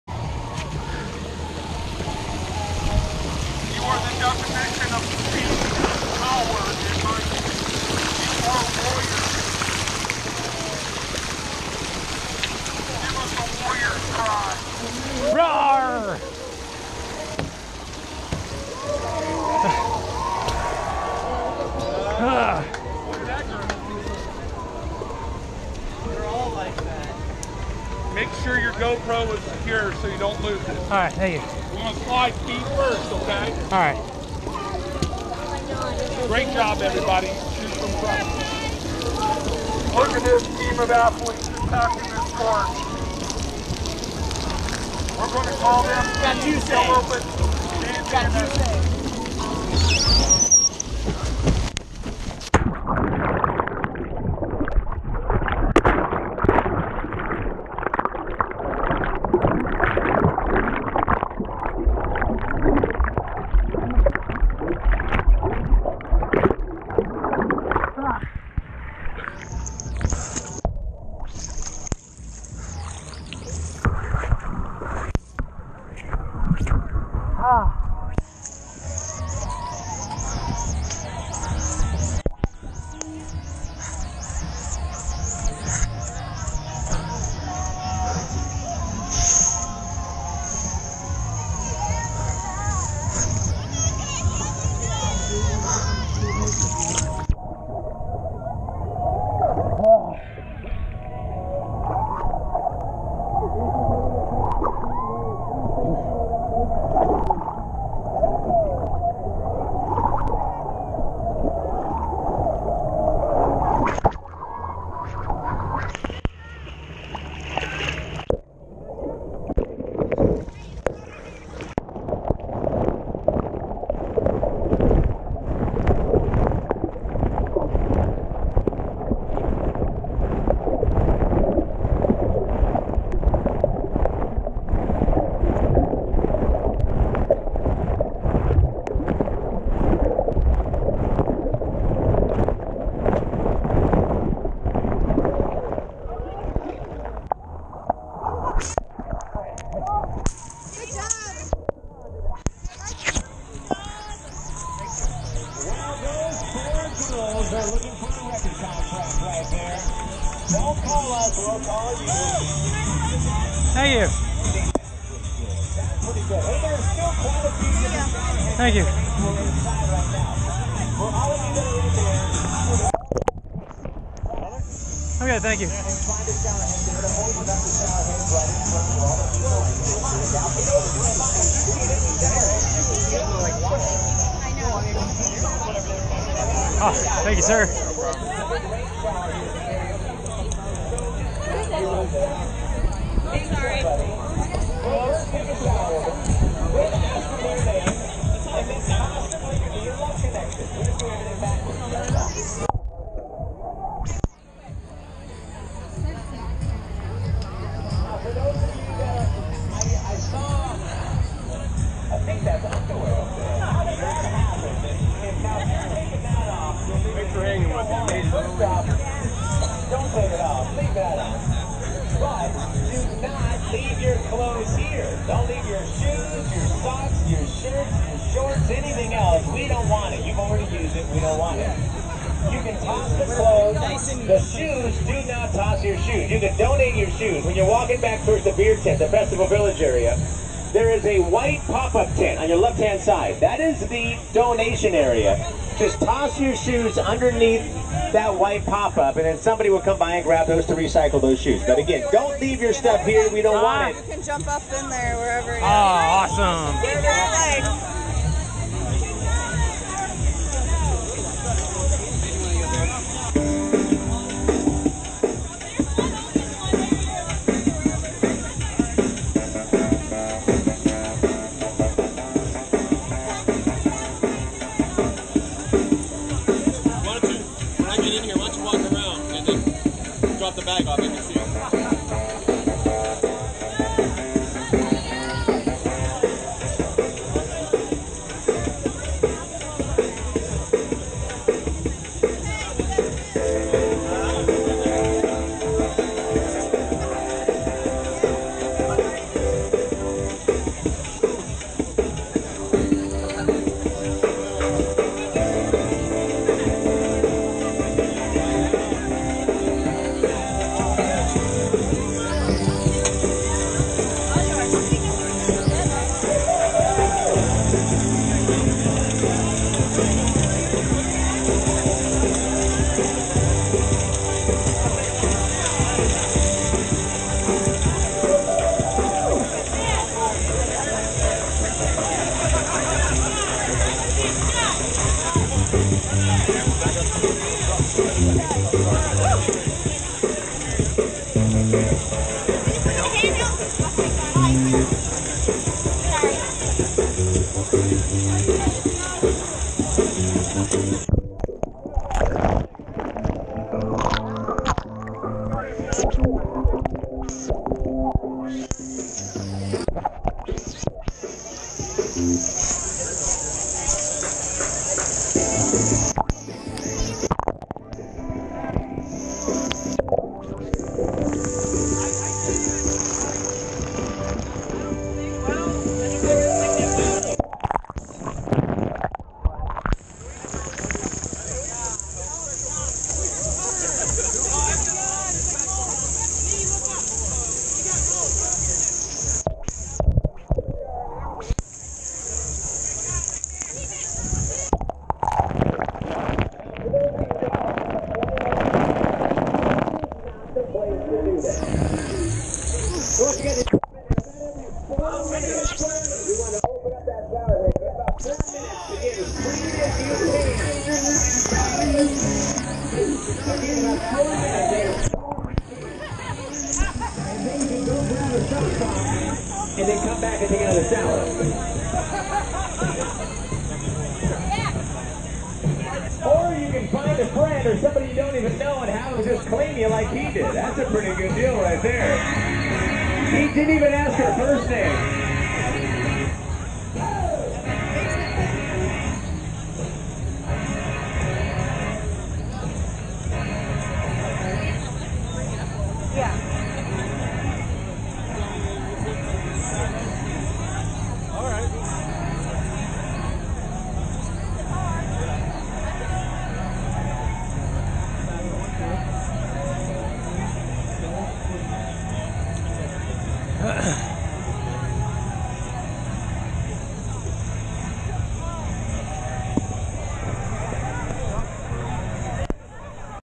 Warrior Dash Ohio 2016 " Goliath"
Last bit of the Warrior Dash 2016 using my Go Pro for the first time .